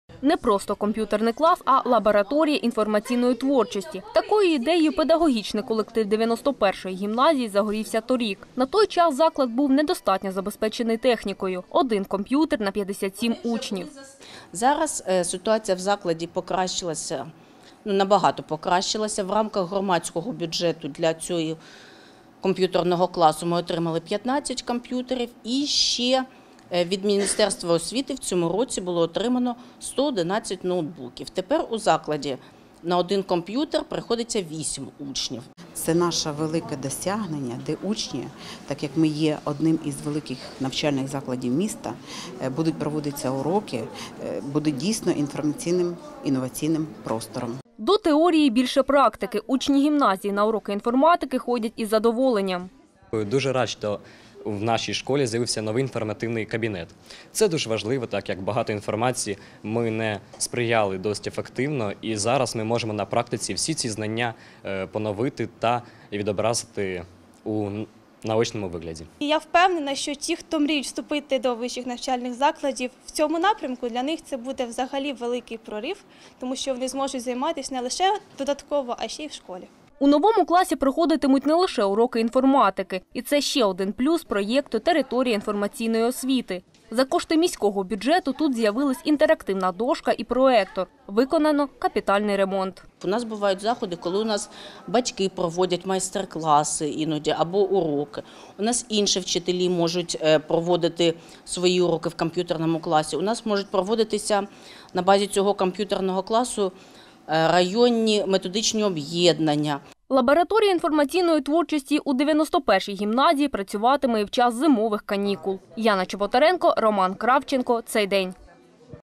Лабораторія інформаційної творчості: новий комп'ютерний клас відкрився в 91-ій гімназії
Лабораторія інформаційної творчості. Втретє 91-а гімназія стала переможцем міського конкурсу «Громадський бюджет». Проєкт з інформаційної освіти гімназистів реалізований цьогоріч. Відтепер уроки інформатики більше як півтори тисячі учнів відвідуватимуть з більшим інтересом. На презентації нового комп’ютерного класу побувала Яна Чоботаренко.